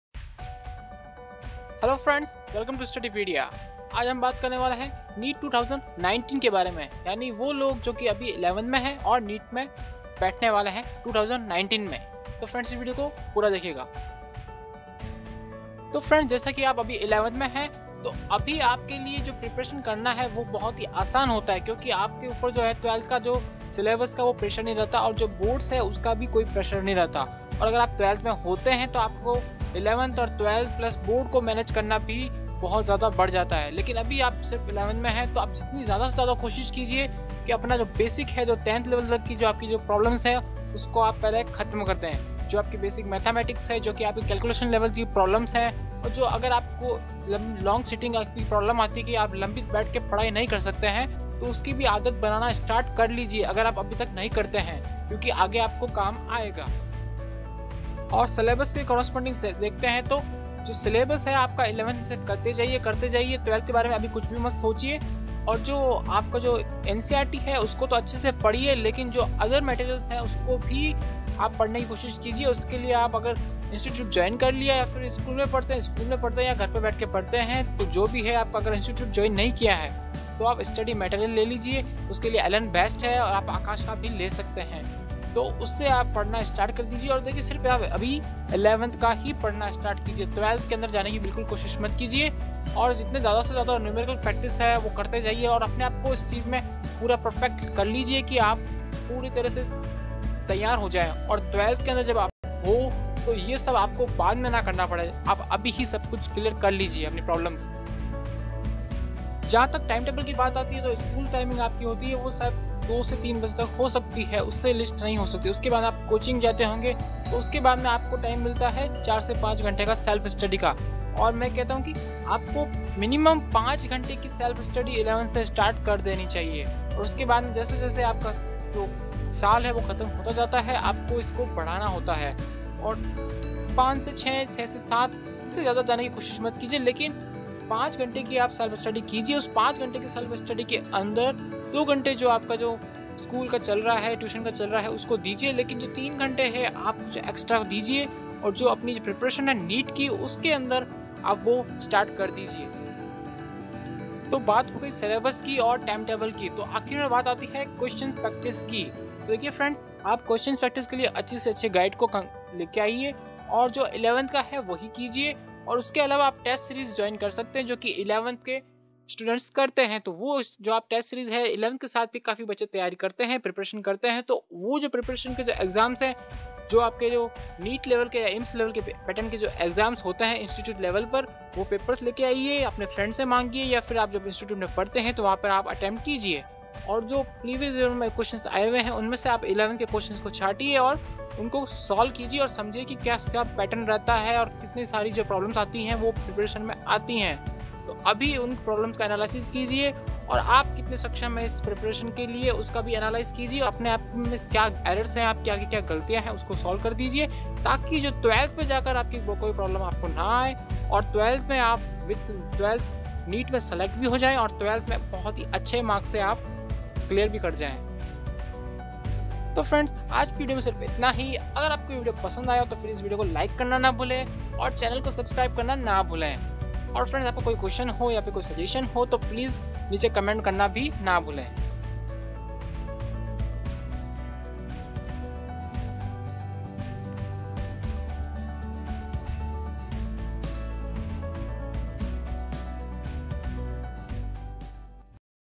Mission NEET 2019 :- For 11th Aspirants | How to prepare for NEET with 11th
Hello friends welcome to Studypedia
Iss video me mane bataya he ki NEET 2019 / NEET ki preparation 11th ke sath kaise kare. Or kase study manage kare, time table kya ho or questions related problems, study Material and test series konsi join kare or kab join kare ye Sab bataya he.